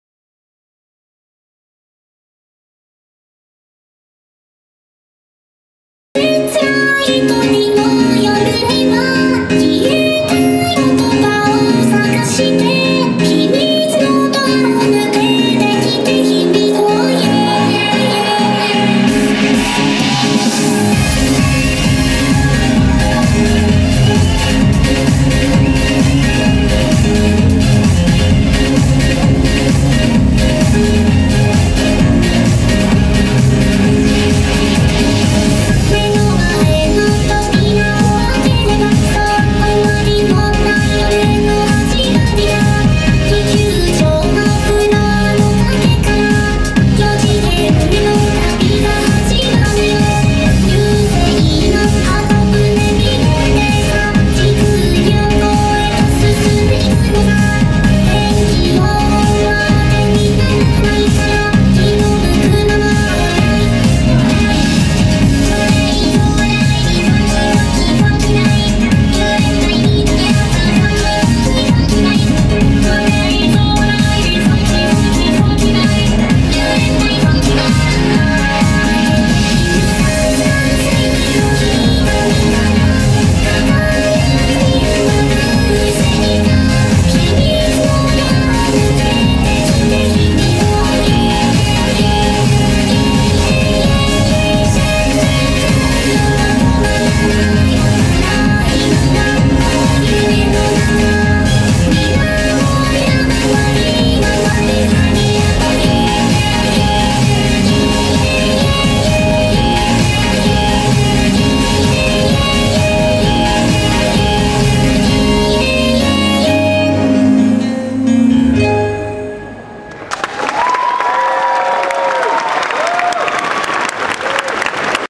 Colossalcon 2014 - Skit4
Colossalcon 2014 Cosplay Competition – Skit4

Colossalcon is an anime convention that was held from June 5th thru 8th at the Kalahari Convention Center and Waterpark Resort in Sandusky Ohio.